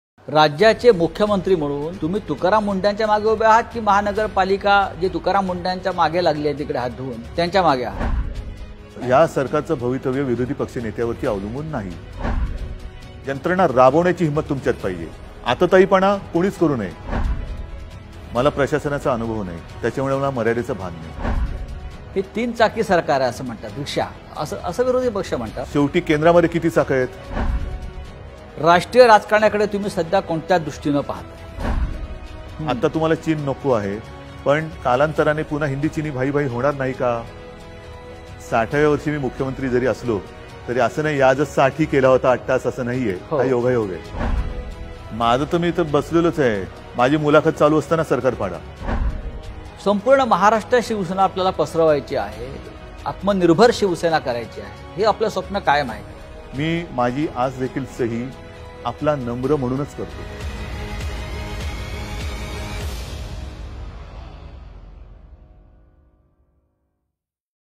CM uddhav Thackeray Interview. Promo - 3 | Sanjay Raut | Saamana
हिंमत असेल तर सरकार पाडा !
मुख्यमंत्री उद्धव ठाकरे यांची दिलखुलास मुलाखत. प्रोमो - 3
CM UddhavThackeray Interview. Promo - 3